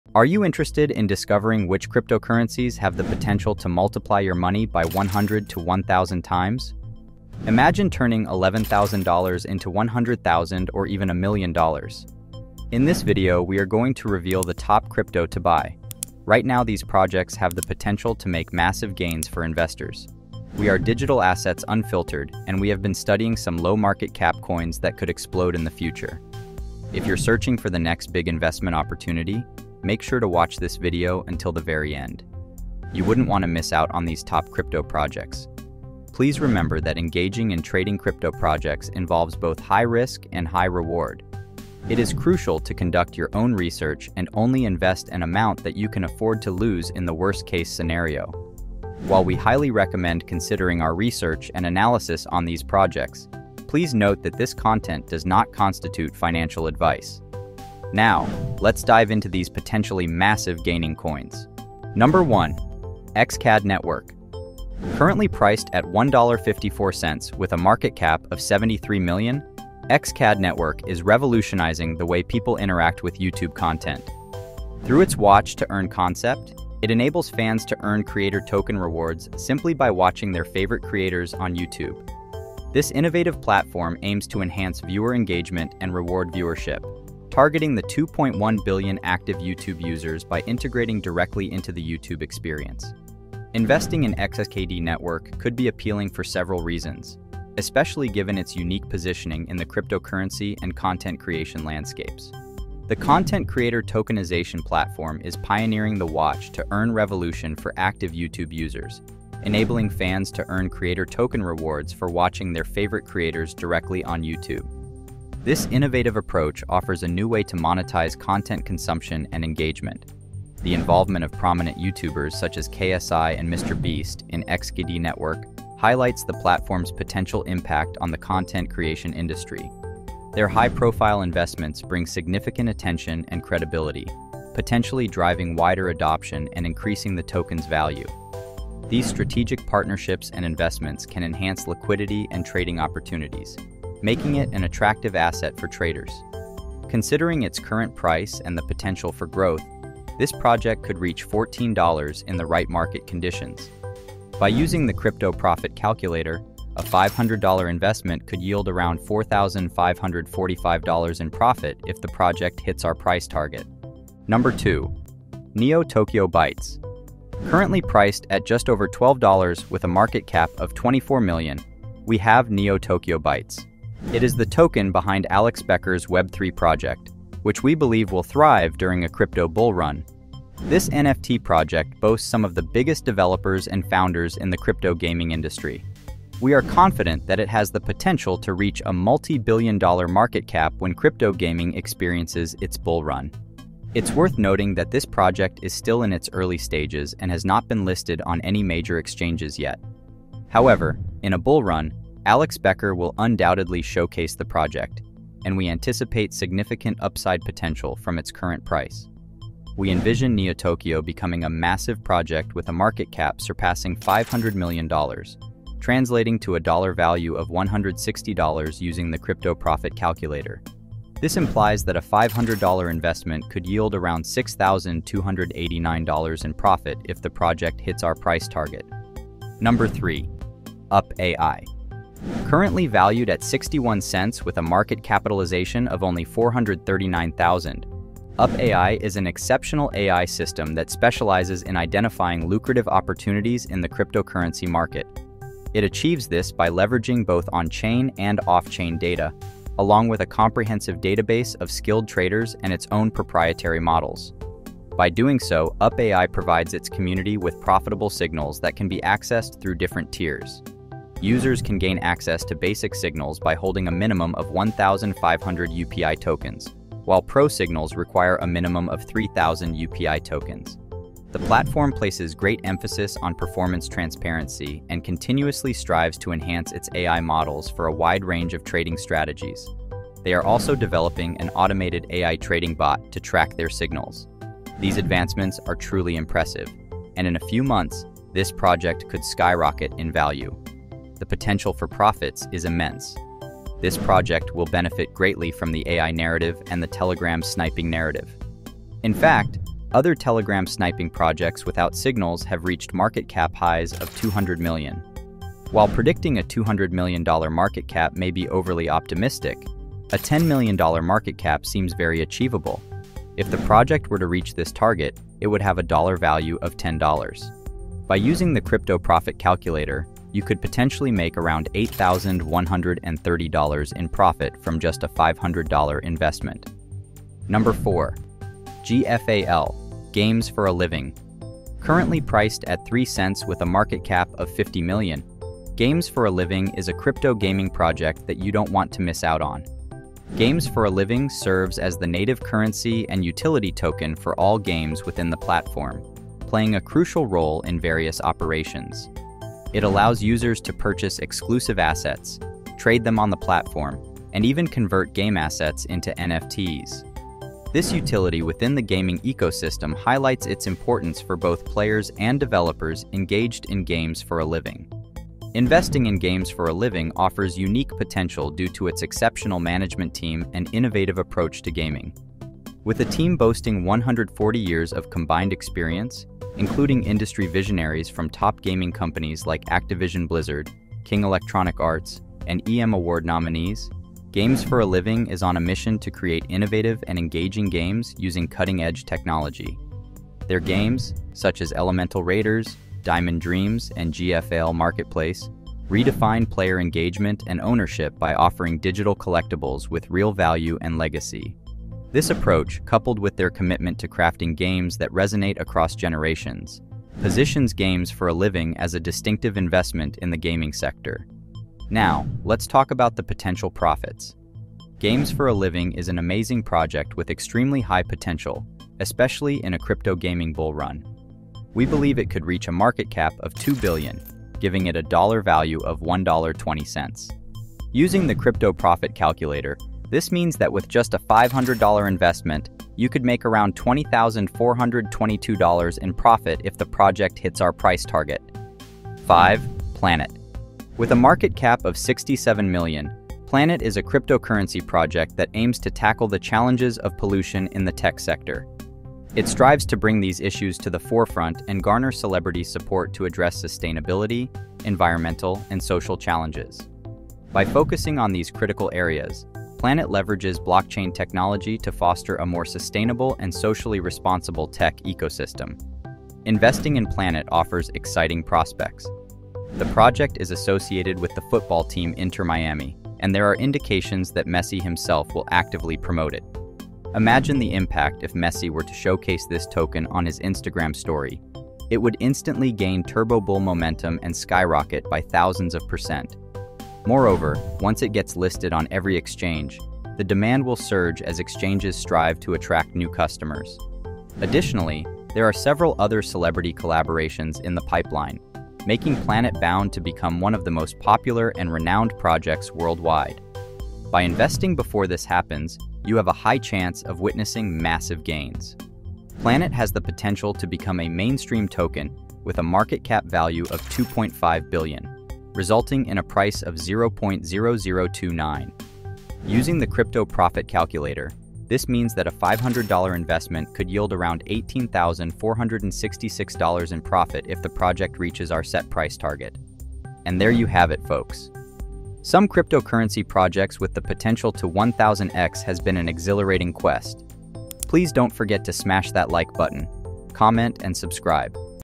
These Crypto Coin GEMS Have 100x to 1000x Potential
Get in early on these crypto coins that have the potential to skyrocket 100x to 1000x! Don't miss out on these profitable opportunities in the world of cryptocurrency. Watch this video to find out which coins have the potential to make you big profits in the future.

⏰Timestamps⏰
00:00 Introduction
01:04 Xcad Network
02:44 Neo Tokyo Bytes
03:50 UPAI
05:27 GFAL- Games For a Living
07:19 Planet